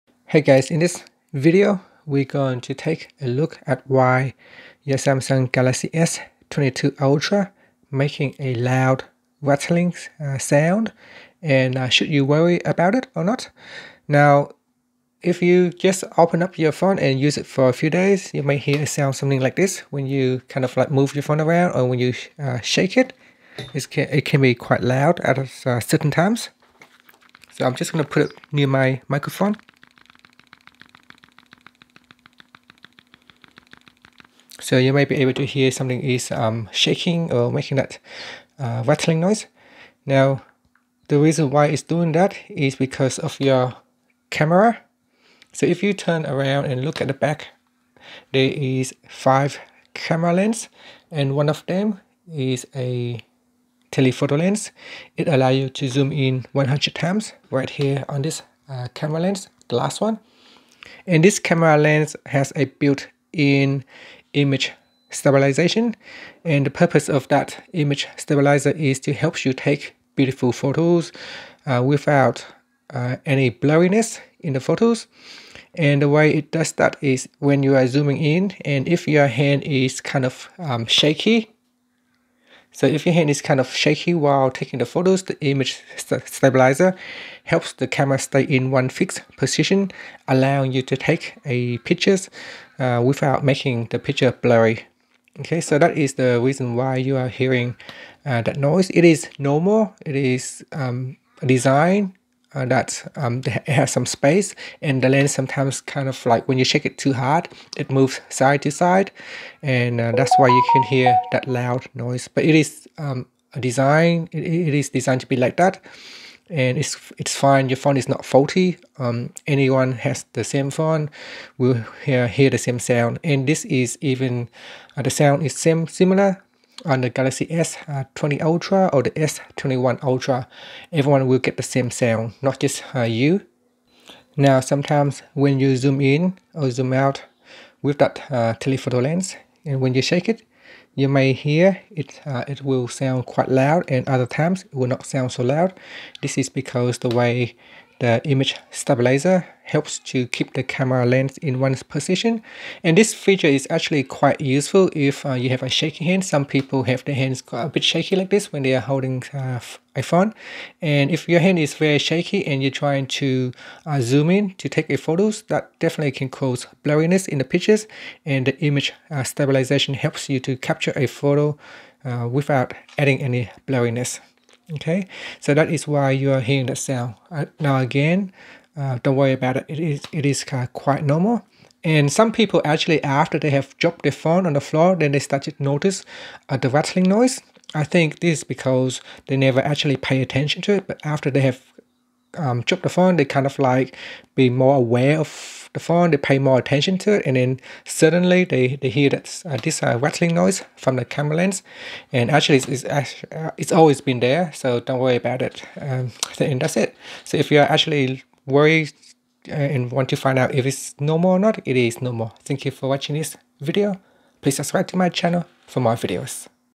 Galaxy S22 Ultra: Loud Rattling Noise - Should You Worry?
See why your Samsung Galaxy S22 Ultra makes a loud rattling sound, and should you worry about it.